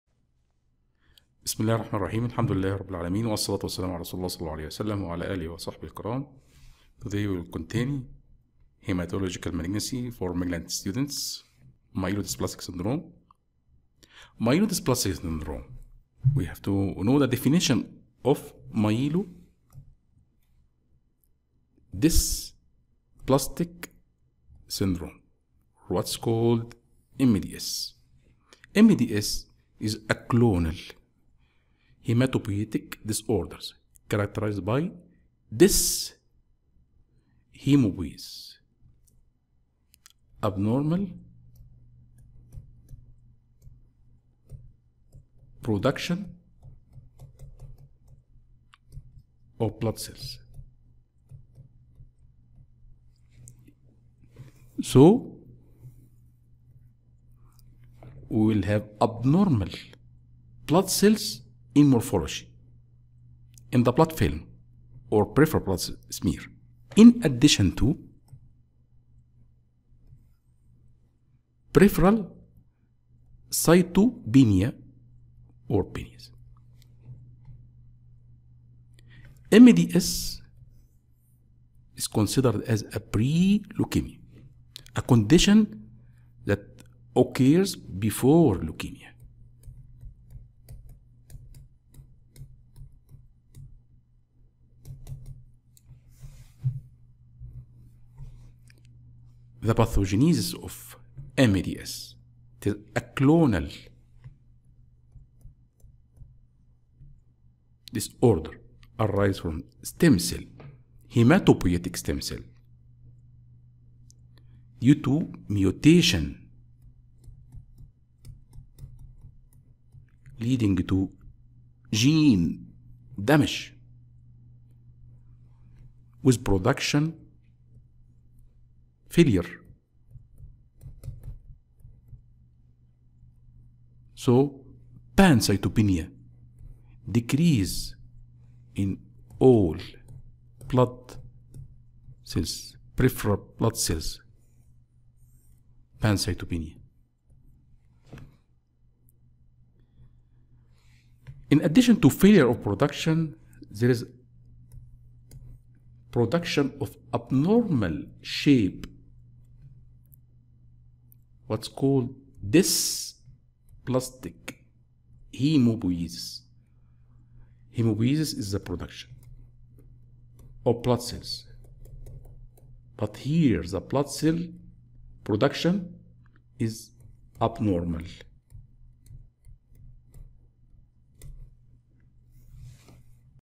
Pathology Hematology | 22 | Myelodysplastic Syndrome (MDS)
الوحدة 1 : Hematological Malignancies
الدرس 22 : Myelodysplastic Syndrome (MDS)
الهدف من الدرس :
Define MDS
Discuss the pathogenesis of MDS